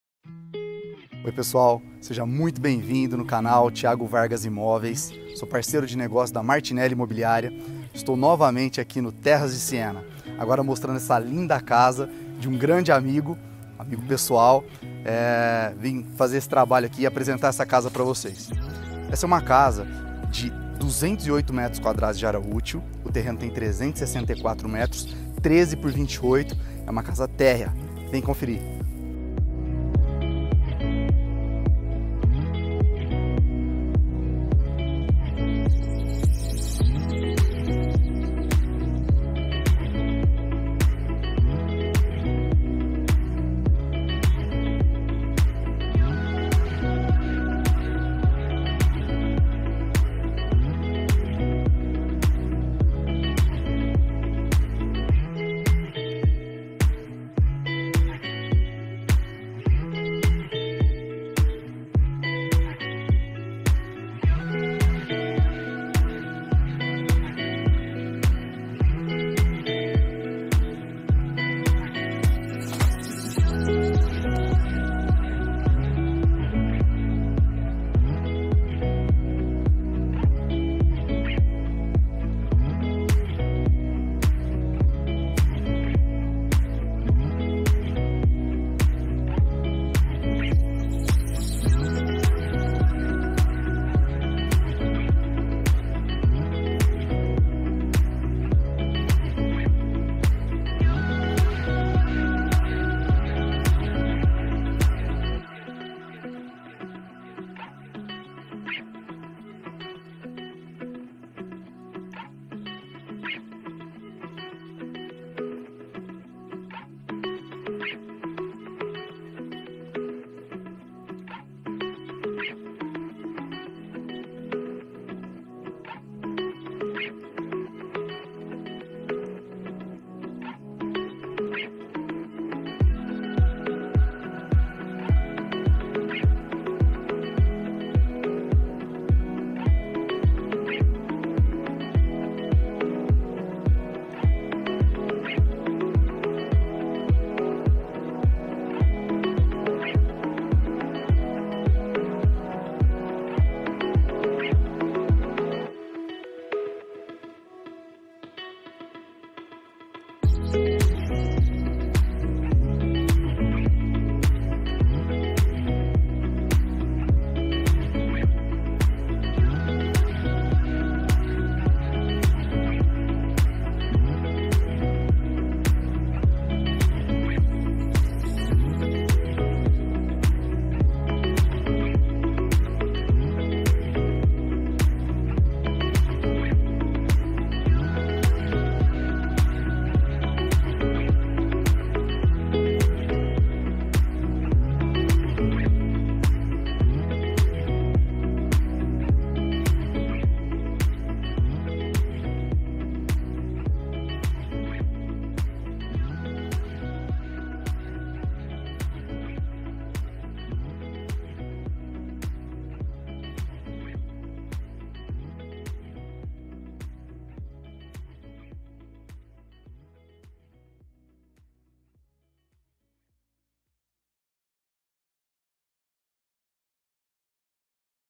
Magnifica Casa-03 Suítes-Condomínio Terras de Siena
03 dormitórios/suítes, terreno de 364m², 208m² de construção, lavabo, cozinha e espaço gourmet integrados, fino acabamento, espaço para churrasco e completa de armários e iluminação,04 vagas de garagem, sendo 02 cobertas.
Valor - R$980.000,00